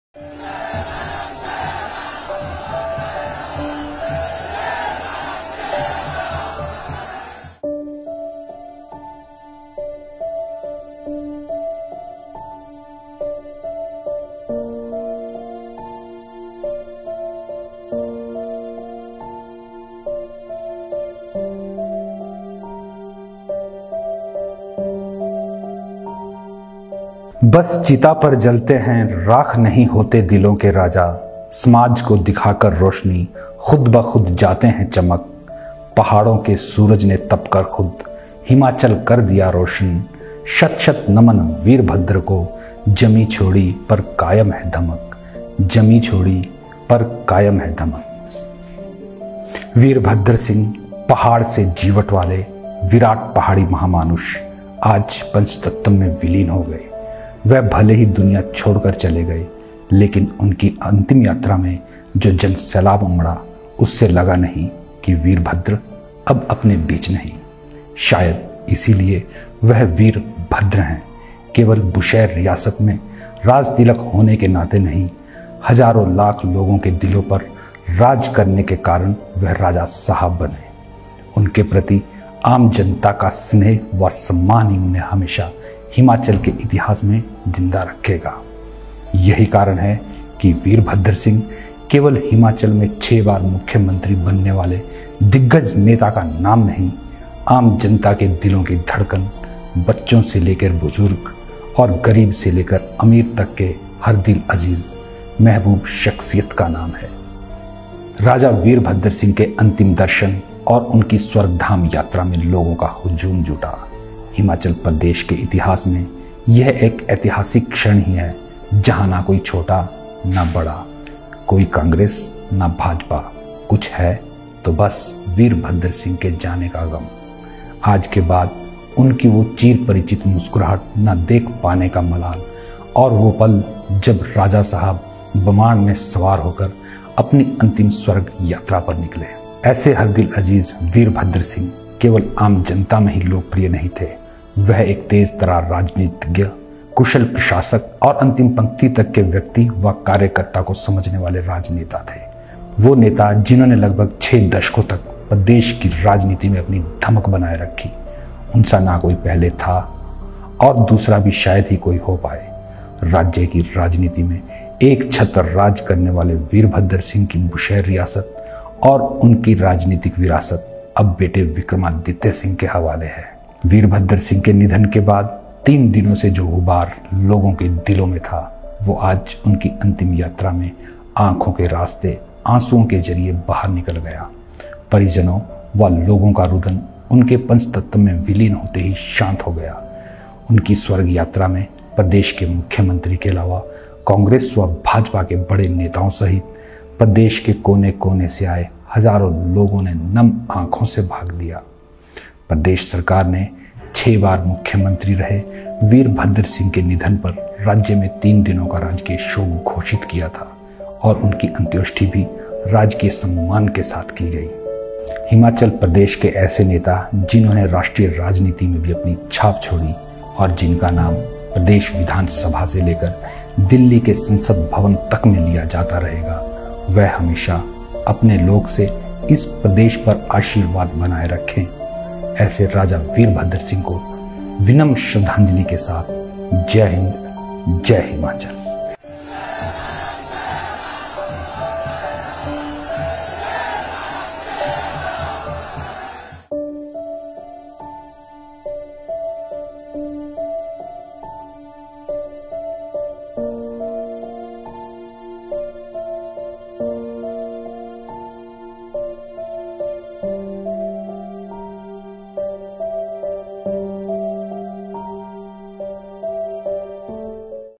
बस चिता पर जलते हैं, राख नहीं होते दिलों के राजा..........
बस चिता पर जलते हैं, राख नहीं होते दिलों के राजा. लाइव टाइम्स टीवी की ओर से राजा वीरभद्र सिंह को भावपूर्ण श्रद्धांजलि